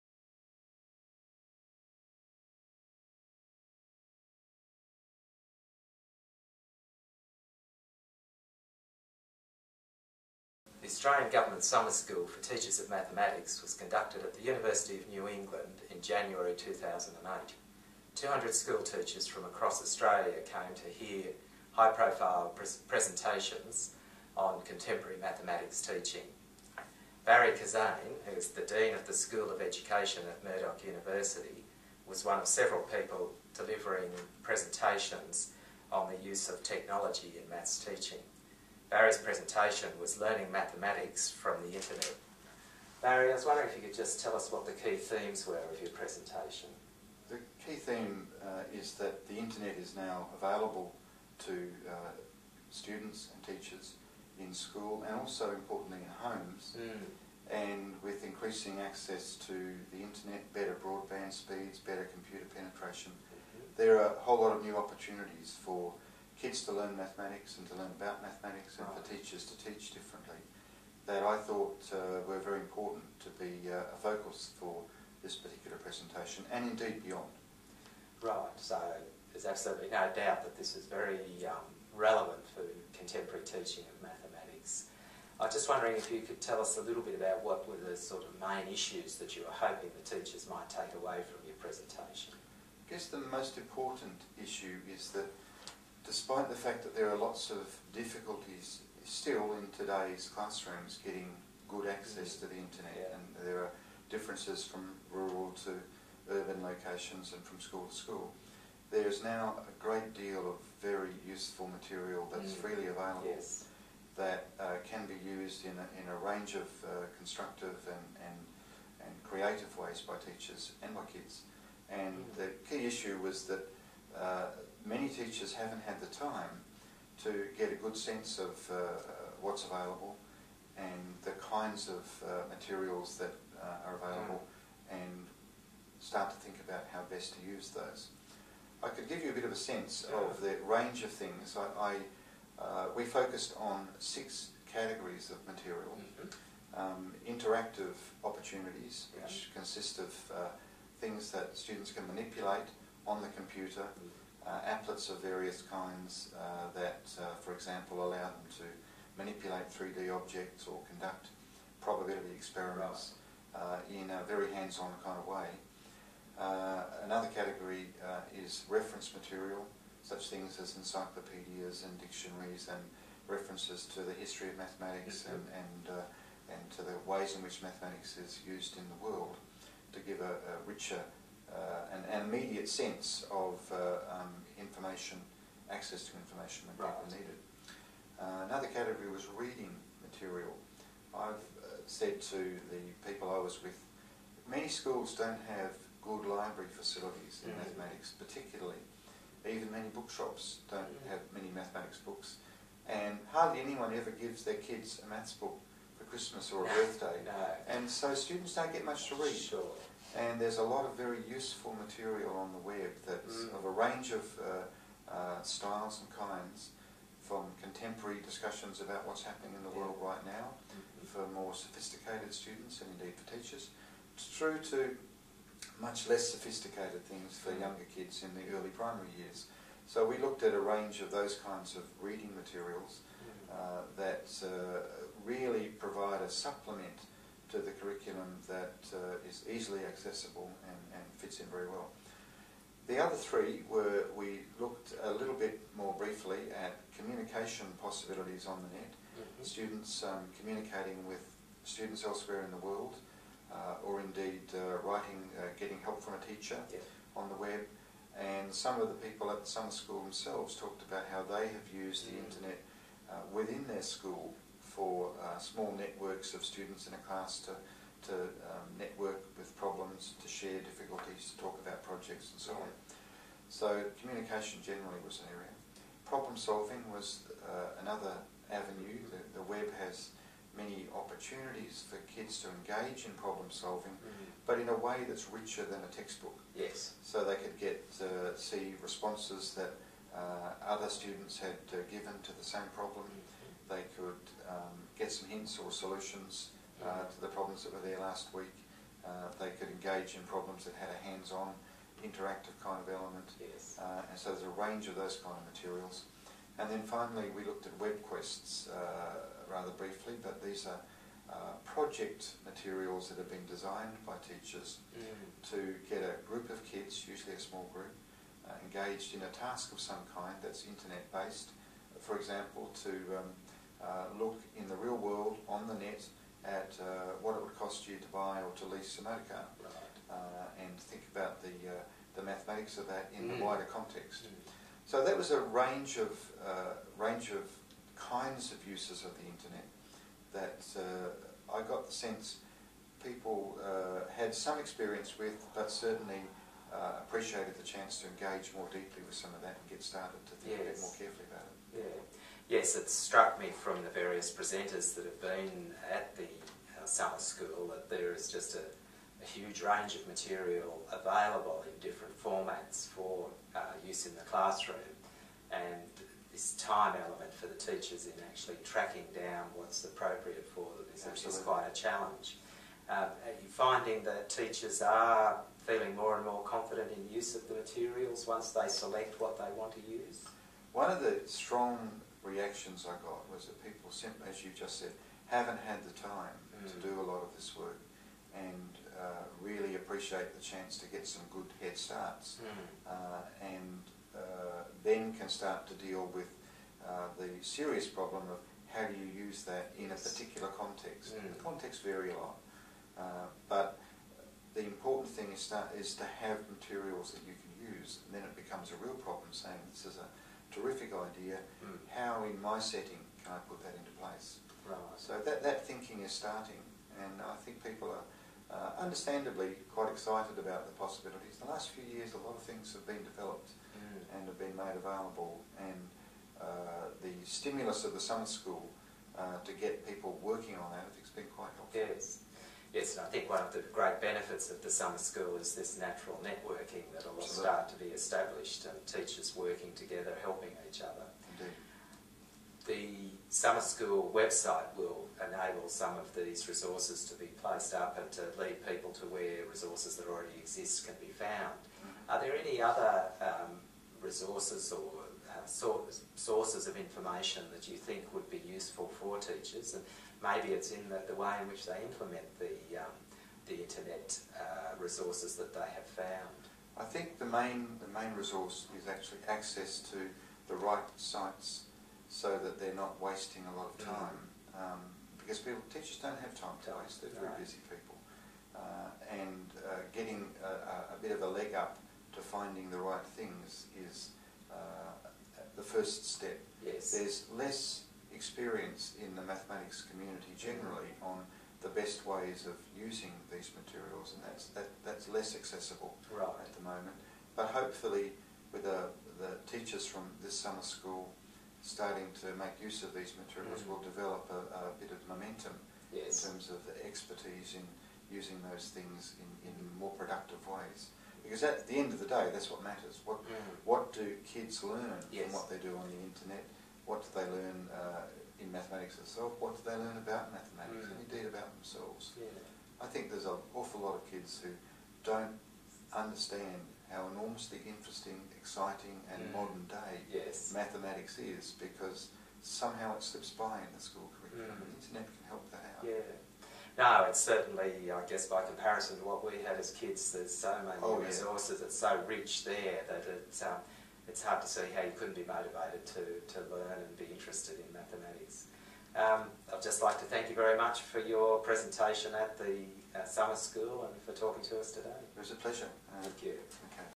Barry Kissane Mathematics Summer School
Barry Kissane talks about learning mathematics via the internet.